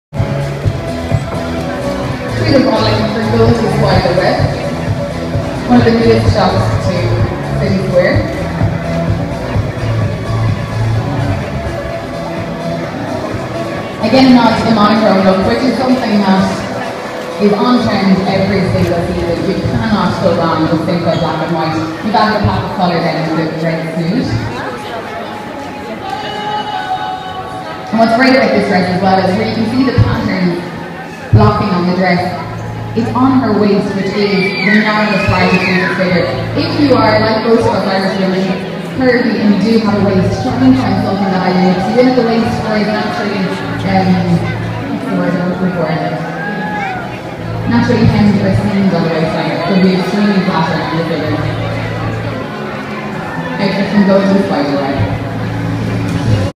Waterford Fashion Show - Alphabet Dress
Our beautiful Alphabet Dress at the Fashion Show in City Square Shopping Centre, Waterford.

Shop Online with Next Day Delivery Anywhere in Ireland!

Boutique Fashion for REAL Women